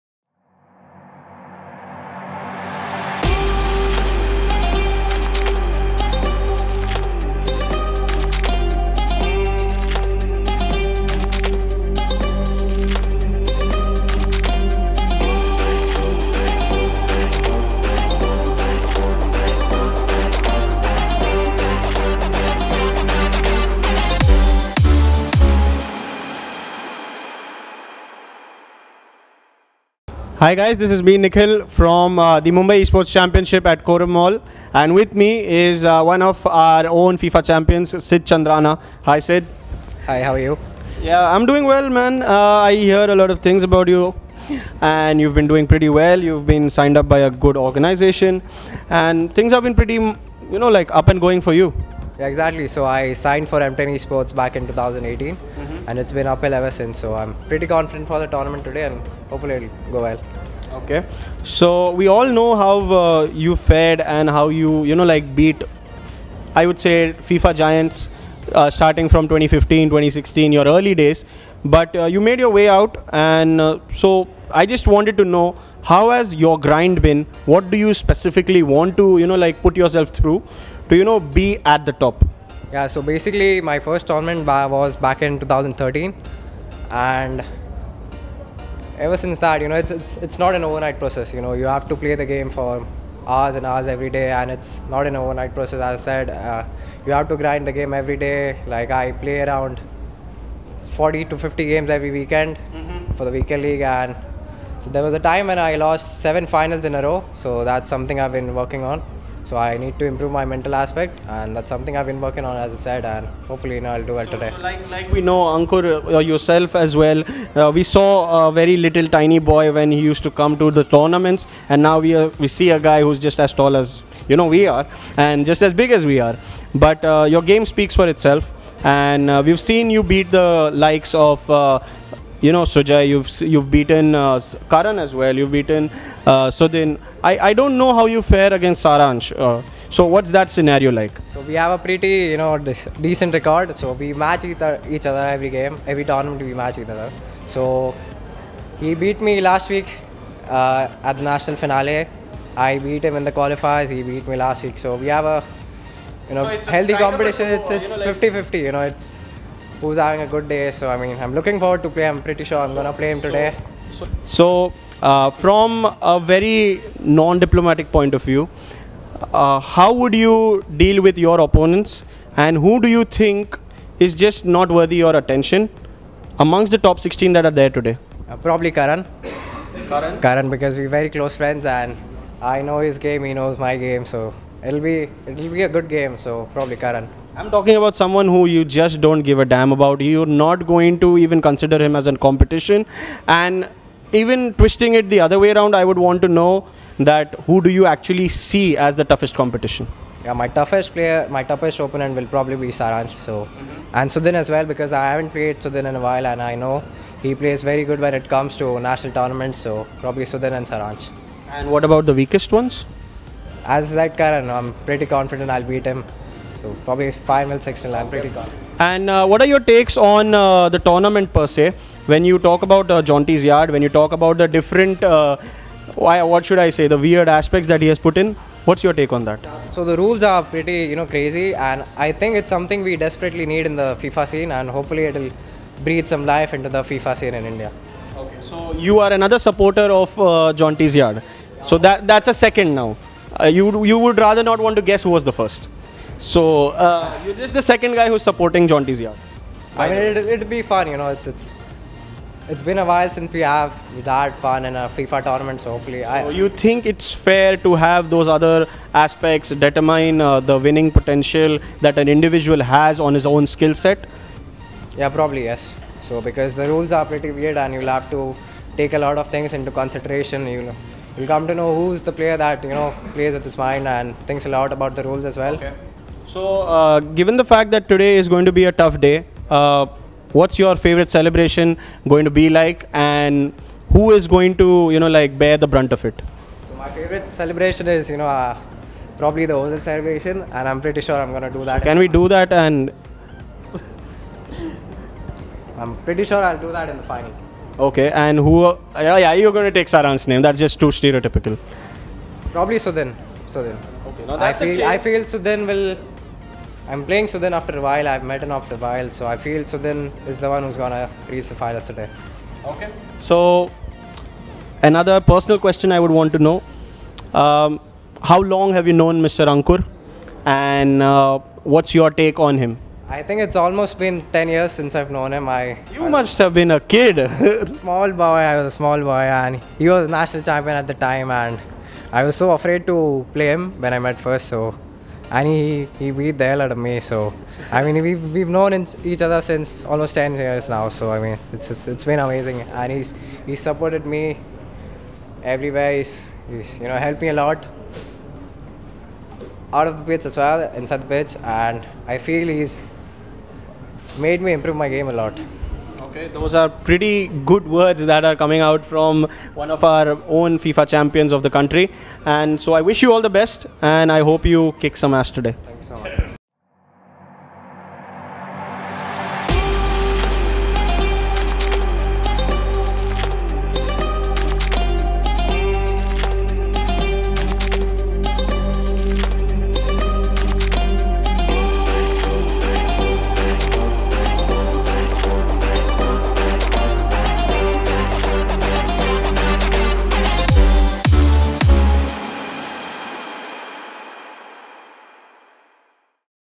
LAN pe Charcha - Siddh 'Jensasid' Chandrana
"It's not an overnight process, you have to play the game for hours & hours, grind the game everyday. I play around 40-50 games every weekend for the weekend league. I have lost seven finals in a row, I have to work on my mental aspect that's something I have been working on."
- Siddh 'Jenasid' Chandarana.